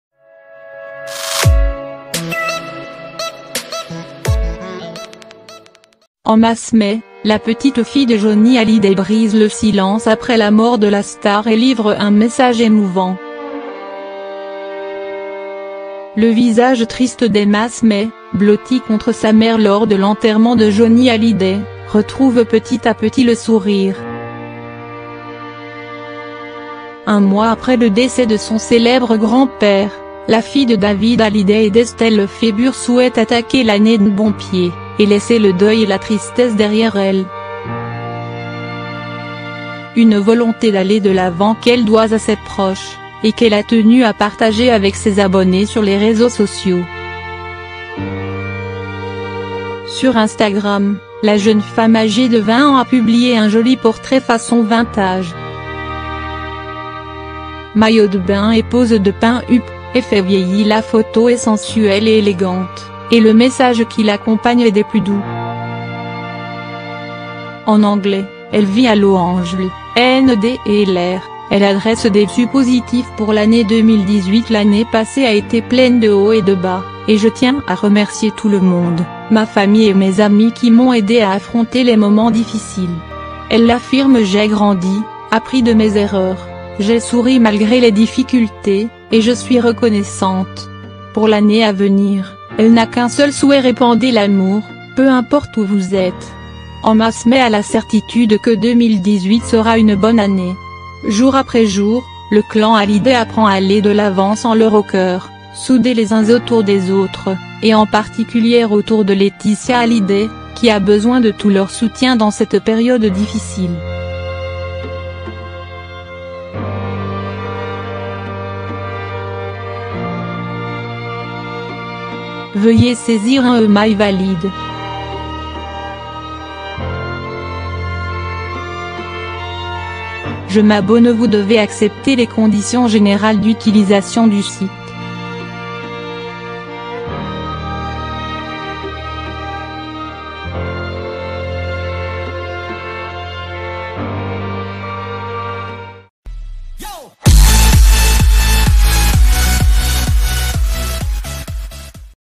Emma Smet, la petite-fille de Johnny Hally­day brise le silence après
Après un diffi­cile mois de décembre, Emma Smet tente de retrou­ver le sourire. La petite fille de Johnny Hally­day a donc adressé des voeux pleins de recon­nais­sance à ses proches pour la nouvelle année.